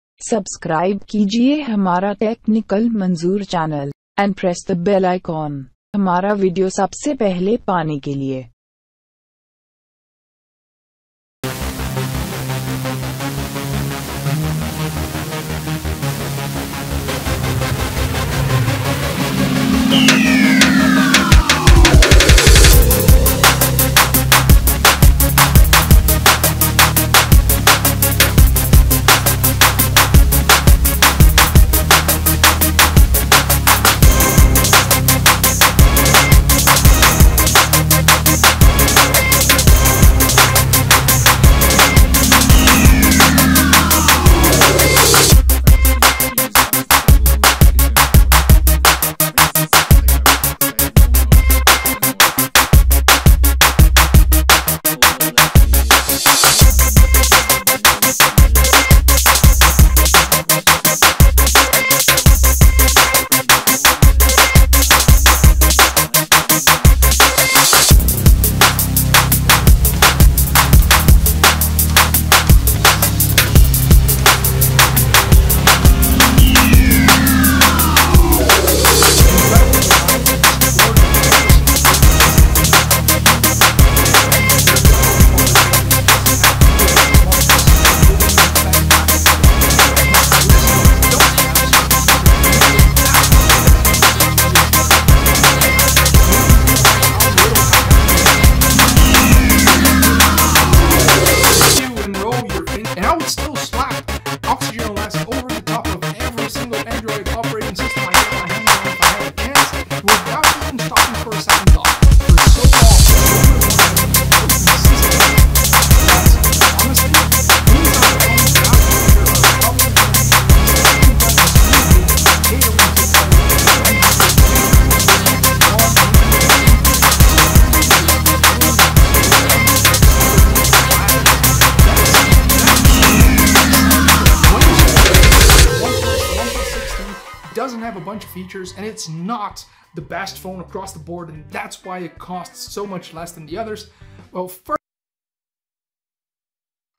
OnePlus 7 First look | Dual Selfie Popup Camera
This videos show OnePlus 7 Feature & First look.One Plus 7 & Dual Selfie Popup Camera available.
One Plus 7 Feature & Fast look
OnePlus 7 - FIRST REAL LOOK!!!